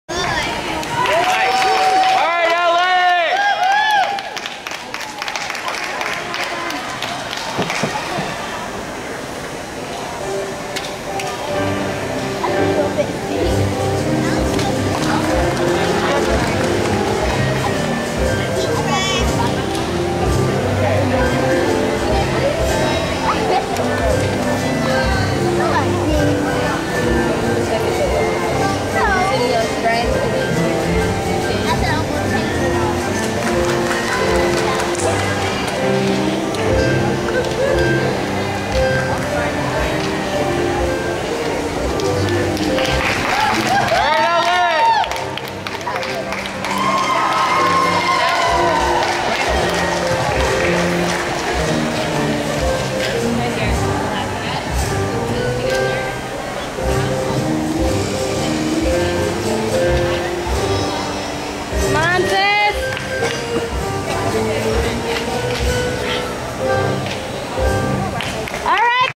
level 4 floor routine at summer nationals espn wide world of sports in disney 2013.
alissa in florida 6/14/13